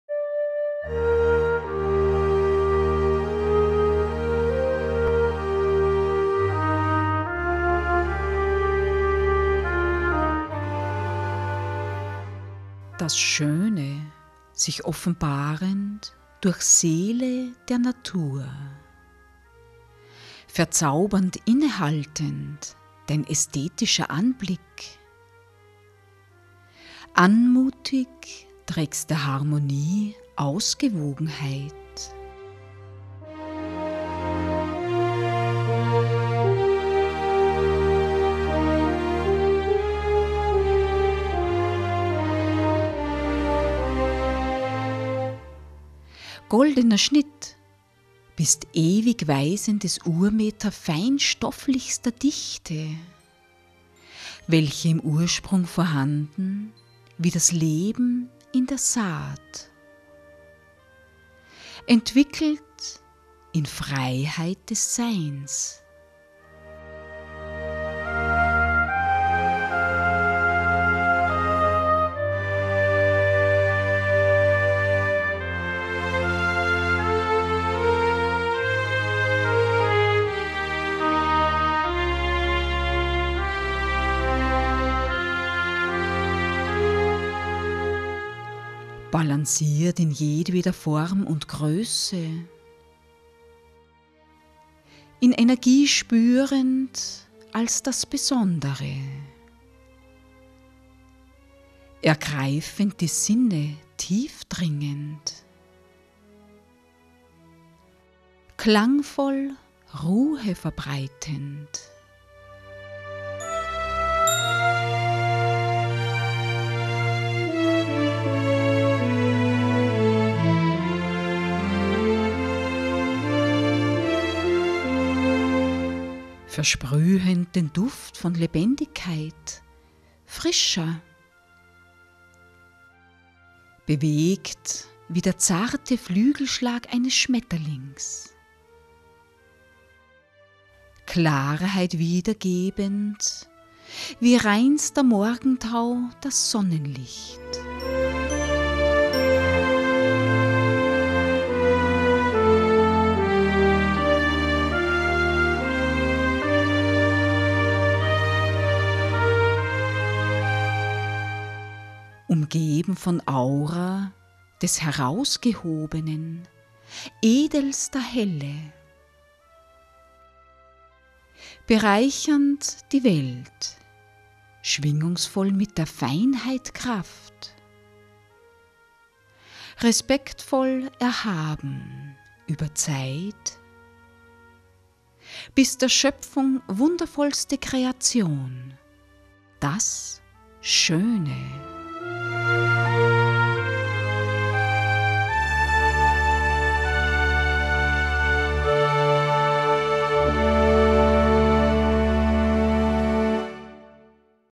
Ulrike Muehlbachler Lesung: DAS SCHÖNE Musik: Prof. T.C. Pfeiler - AKM Color Lyrik Entspannung relax
ULRIKE MUEHLBACHLER
Lesung: DAS SCHÖNE
Musik: Prof. T.C. Pfeiler - AKM
Lyrik zur Entspannung
Tiefgründige und tiefsinnige Gedanken, vorgetragen von der Autorin selbst, wofür sie eine eigene Kunstsprache entwickelt hat.

Die komplette Instrumentalmusik dazu von Prof. T.C. Pfeiler:
Prof. T.C. Pfeiler mit dem Vienna Symphonic Library (VSL) Bigbang Orchestra, Elite Strings  & Smart Orchestra
Aufgenommen ím Februar 2022 in Seekirchen am Wallersee, Österreich

Eingestellt bei Youtube am 13. März 2022